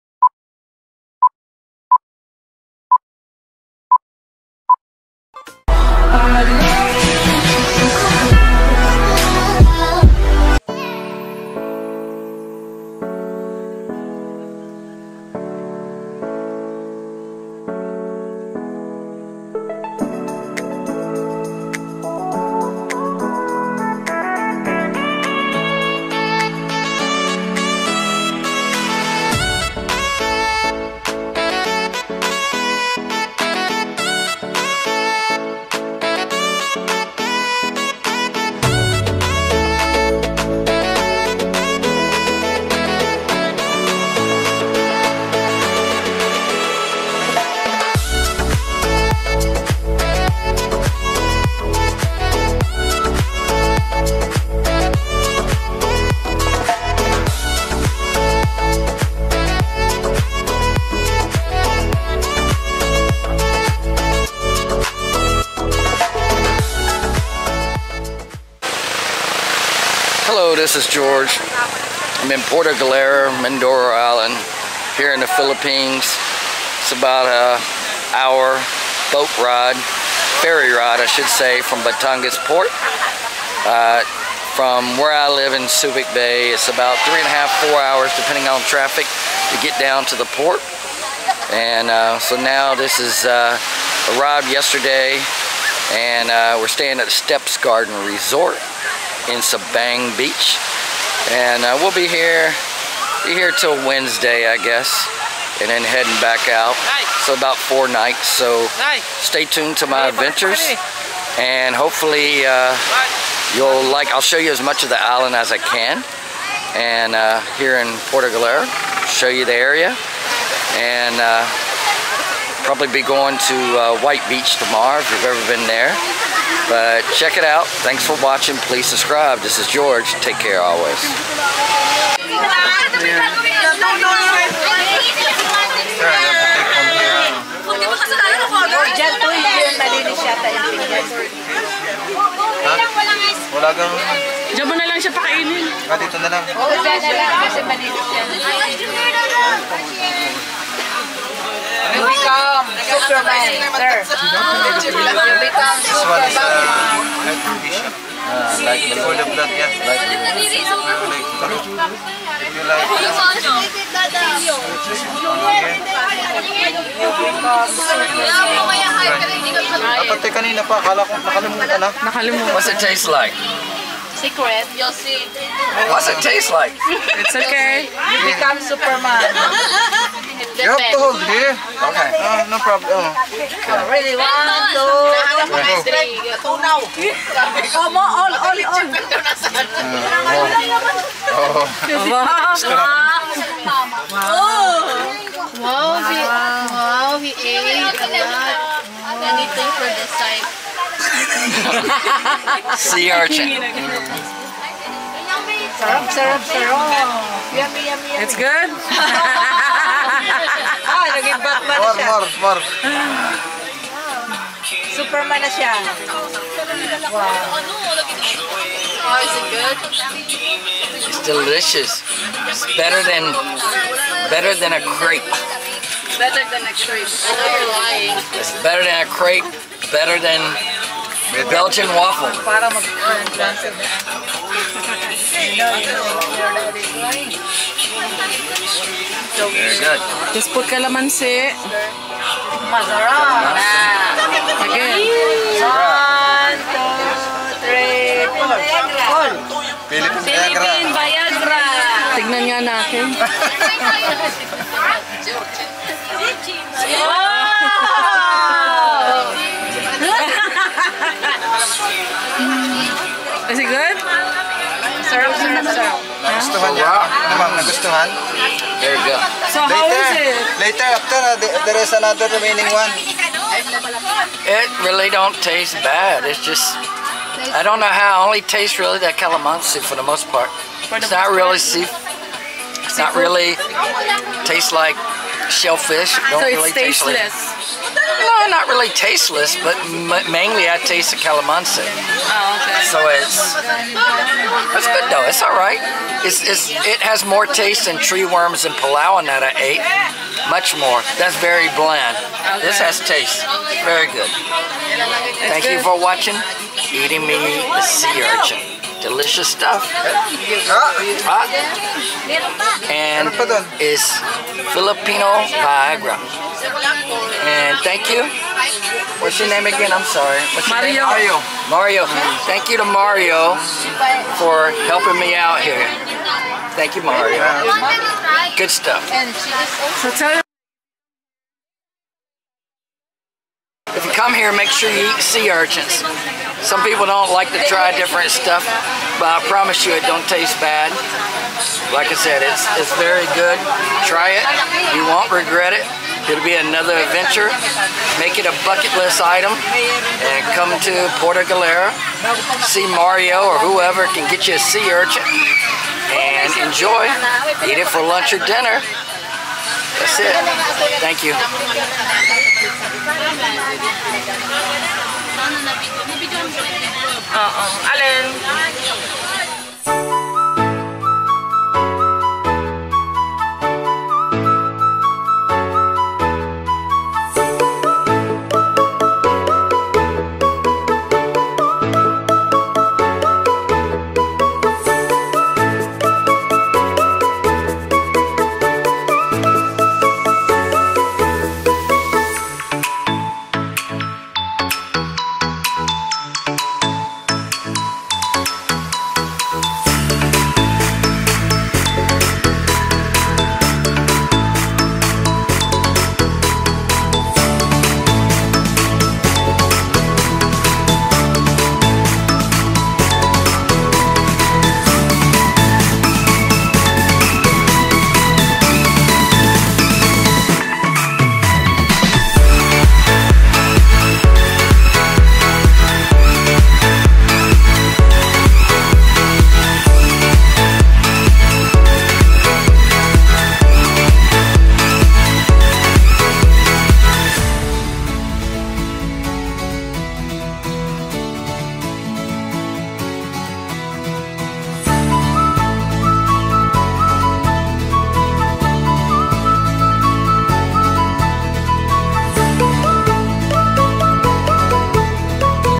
Eating Sea Urchins in Puerto Galera
Join me in eating Sea Urchins in Puerto Galera. I went island hopping yesterday and after a delicious BBQ beach lunch, I had an opportunity to try Sea Urchins. They were not bad, but after putting Calamansi juice on them, thats mainy what I tasted. They did have more flavor then tree worms in Palawan. Tree worms were just bland & very slimy. Supposedly Sea Urchins are like Balut in that they are filipino viagara. The tagalog name for sea urchin is salungo. The Gonads are the edible part of the sea urchin (animal) that contains sex cells and gametes. The female which I ate are orange with granular eggs while the males are yellow with creamy yellow sperms. You're welcome. :) lol